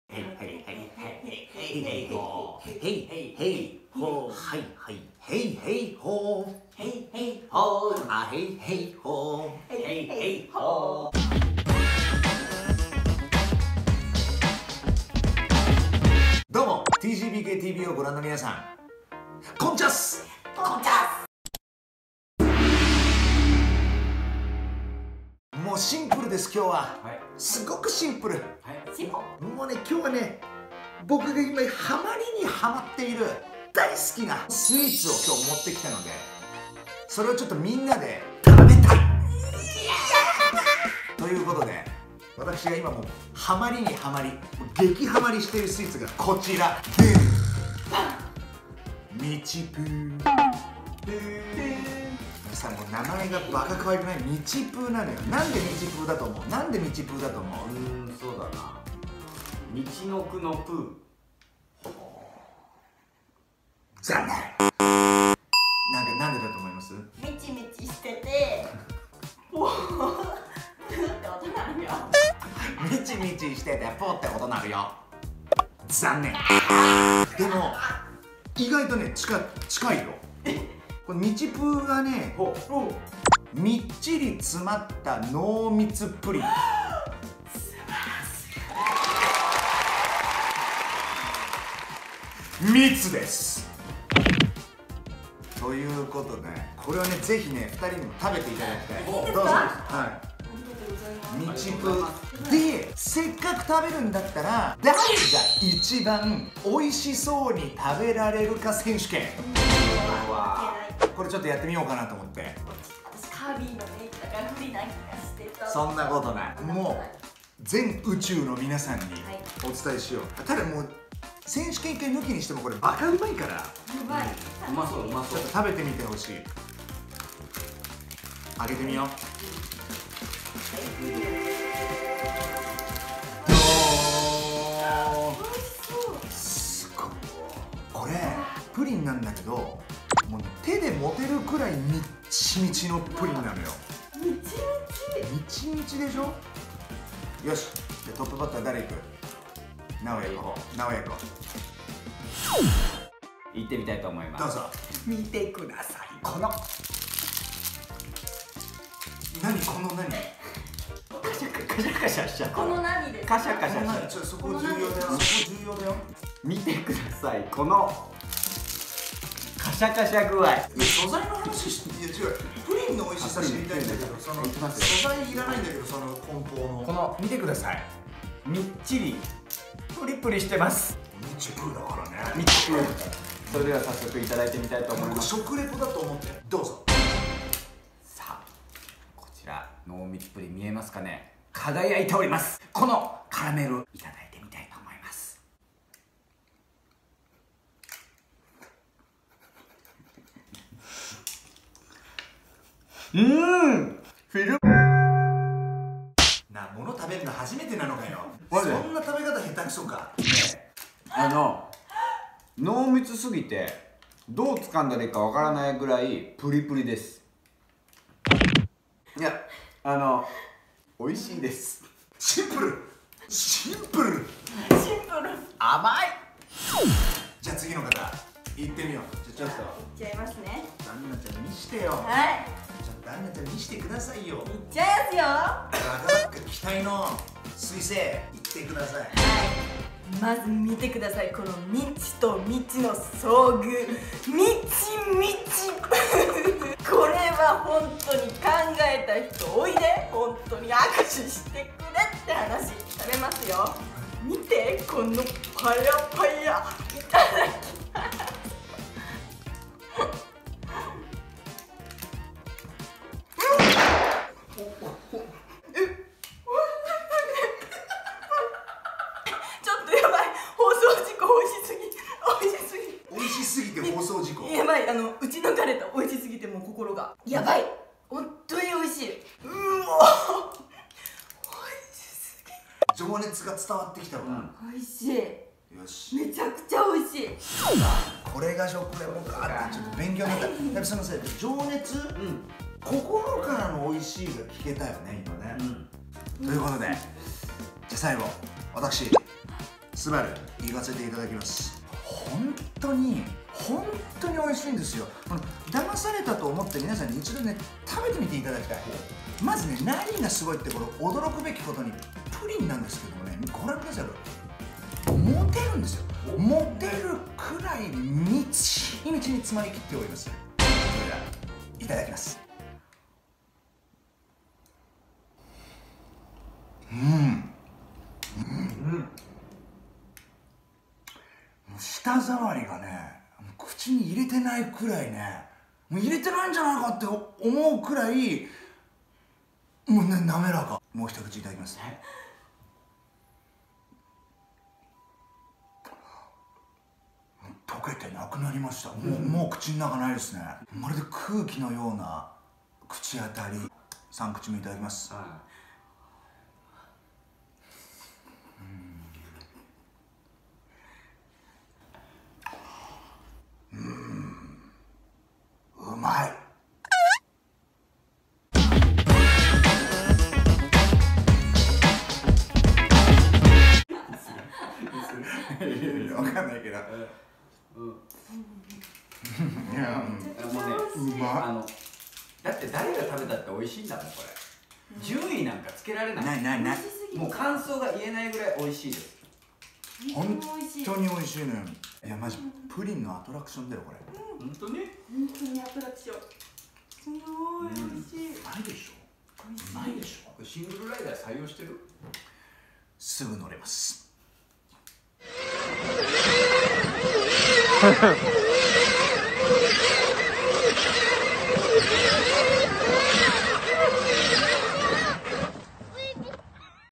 【 TGBKTV 】超オススメの激ウマスイーツ「MICHIPU」を食べる。
超オススメの激ウマスイーツ「MICHIPU」で食レポしてみた動画